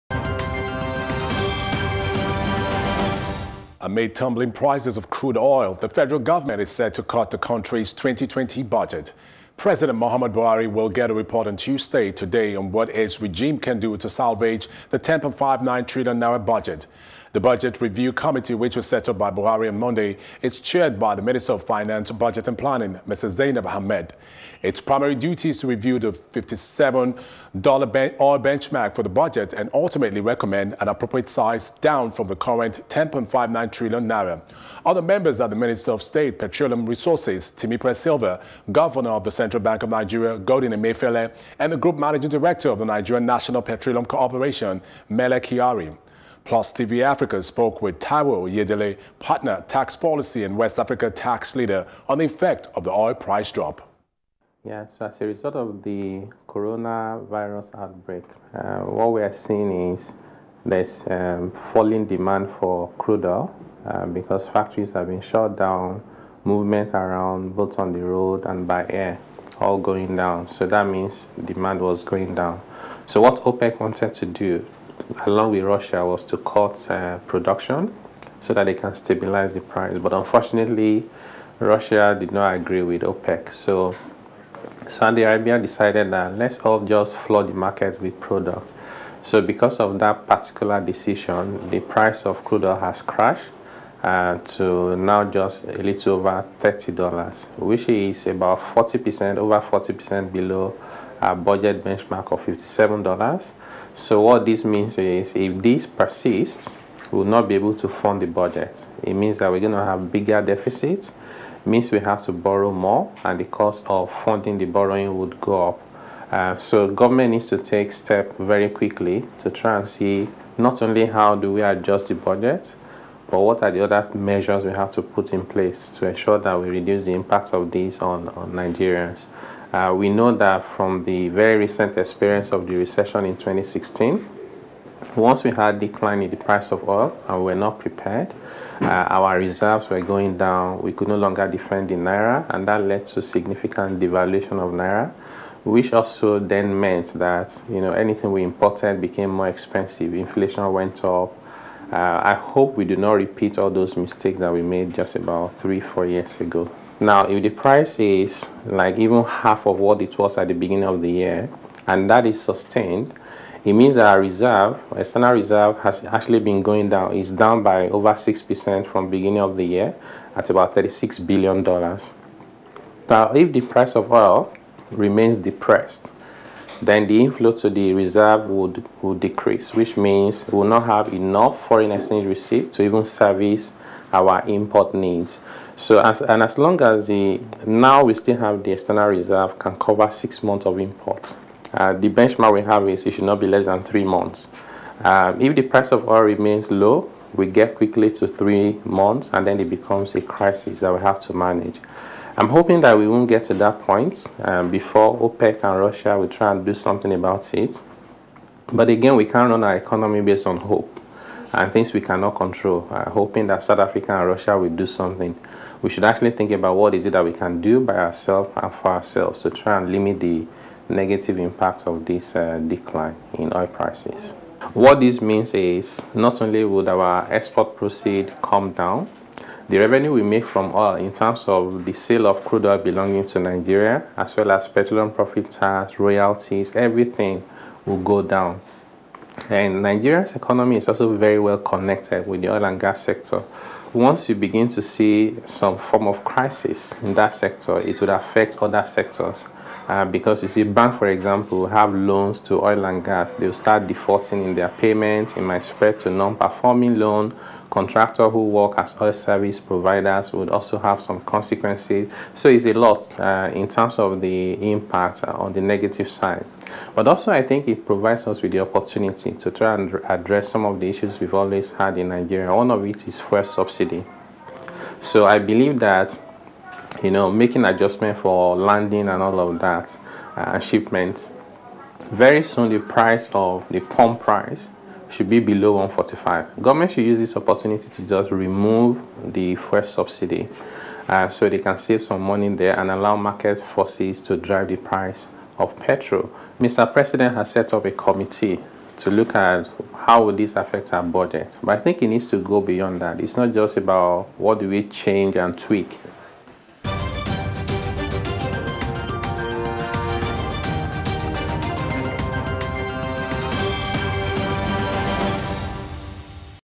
Coronavirus: FG To Cut Budget, Seeks New Oil Benchmark
Amid tumbling prices of crude oil, the Federal Government is set to cut the country’s 2020 budget. President Muhammadu Buhari will get a report on Tuesday (today) on what his regime can do to salvage the N10.59tn budget. The budget review committee, which was set up by Buhari on Monday, is chaired by the Minister of Finance, Budget and Planning, Mrs Zainab Ahmed. Its primary duty is to review the $57 oil benchmark for the budget and ultimately recommend an appropriate size, down from the current N10.59tn. Other members are the Minister of State, Petroleum Resources, Timipre Sylva; Governor of the Central Bank of Nigeria, Godwin Emefiele; and the Group Managing Director of the Nigerian National Petroleum Corporation, Mele Kyari.